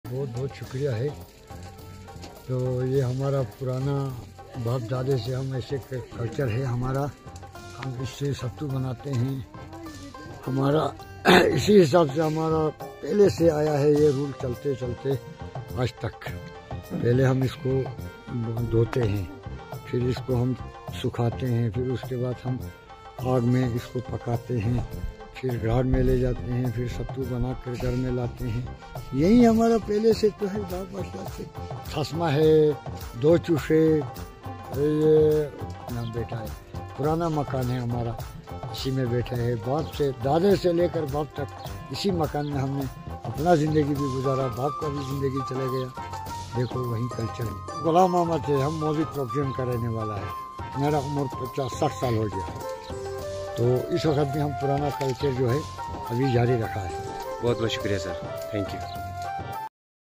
shina traditional culture Gh. Mohd Resident Bearess the tradition, culture since ancestors in Drass
Traditional culture Gh. Mohd Resident Bearess the tradition, culture since ancestors in Drass.

Ghulam Mohammad Resident of Troungen Bearess briefing about the tradition, culture following since ancestors in Drass.

This whole process is called "judo" in Sheena

The video appears to be showcasing traditional practices in the Drass region of Ladakh, related to food, culture, and heritage. The term "judo" in this context seems to refer to a traditional process of preparing and preserving food, particularly "stu" ( possibly a local dish). The speaker, Ghulam Mohd, shares how this tradition has been passed down through generations and is still an essential part of their cultural identity.